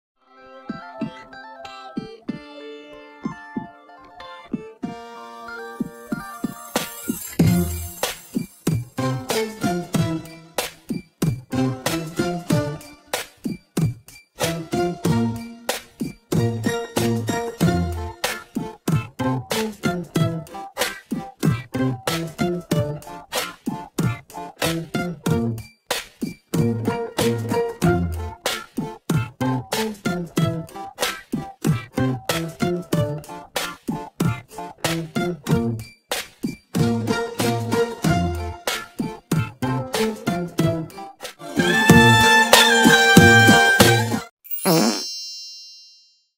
Send this to your SAD FRIENDS with no context
I wanted to do this video for the longest time. This video is inspired by my dearest friends. I have friends who are undergoing a "dark" state in their lives right now. I just wanted to shed some light on the situation and make them smile. Also send this to your random friends hihi

Always remember to smile more, worry less, and LOVE lots!

Xx Chrissa